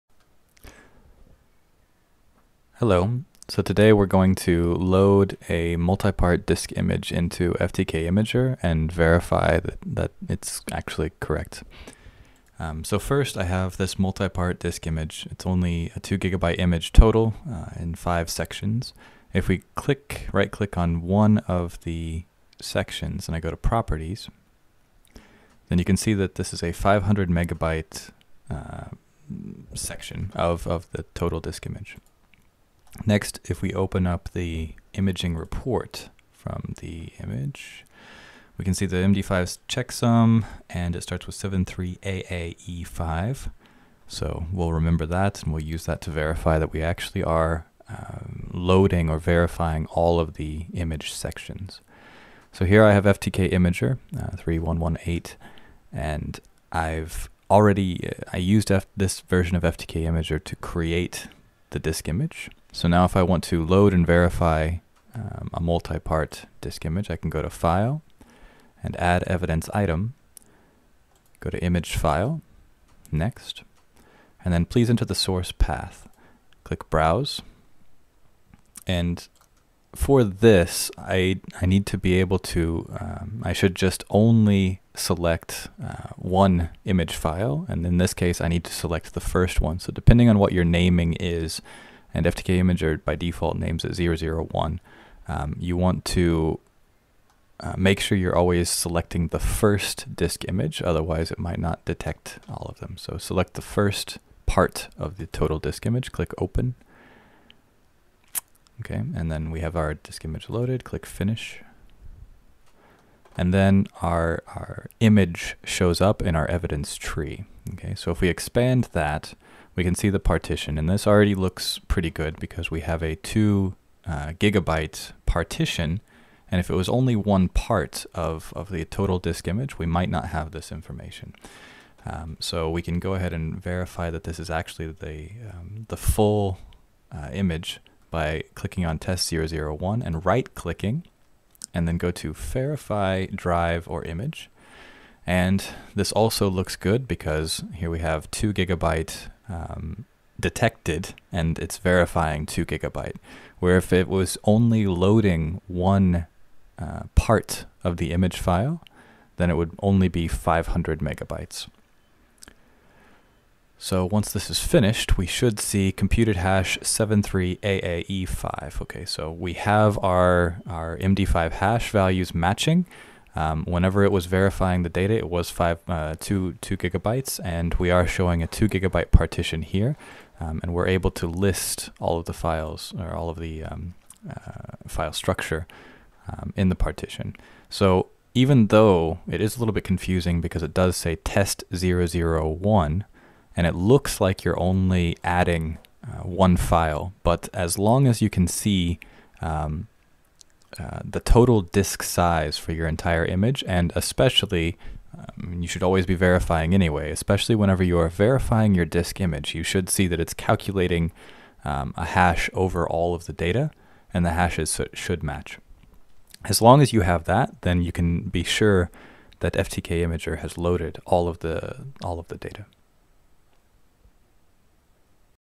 FTK Imager - Loading a multi-part disk image
In this video, we show you how to load and verify a multi-part disk image in FTK Imager. FTK Imager from AccessData can be downloaded for free

FTK Imager is a disk imaging and basic analysis tool use for digital forensics. It is a relatively easy tool to use, even for beginners.

Forensic disk images are bit-by-bit copies of digital storage devices. We verify the integrity of digital forensic images by calculating a hash value of the data. Hashing algorithms normally used in digital forensic investigations are MD5 and SHA1. Other hash values may be used, depending on the legislation in your country (what your court accepts).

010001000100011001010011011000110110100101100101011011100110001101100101
Help make DFIR tutorials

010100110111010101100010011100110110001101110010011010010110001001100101

Please link back to the original video. We would love to see what you are doing and will probably allow its use.